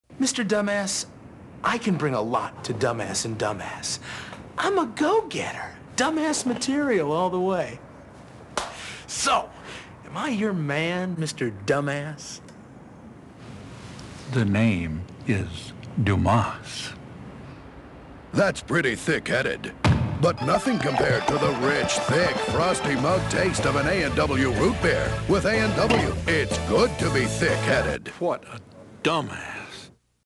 A&W Rootbeer Dumass
A&W Rootbeer
a man is getting a job interview by the boss whose name is Mr. D-u-m-a-s-s. and he keeps calling him Mr. Dumbass. The boss leans over the desk and says, "the name is Dumas." (it's pronounced Dumas, but spelled Dumass.)